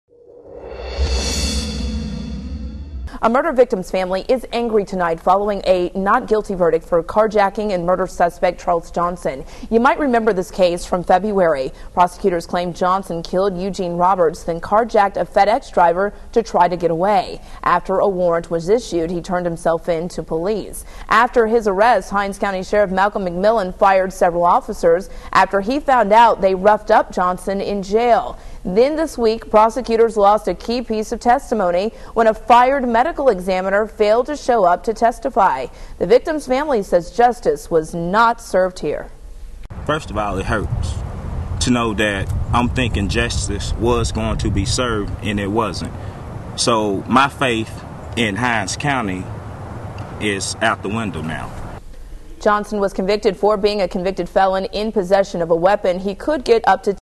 Man Not Guilty Of Murder, Armed Robbery
A Hinds County jury finds a Clinton man not guilty of murder and armed robbery.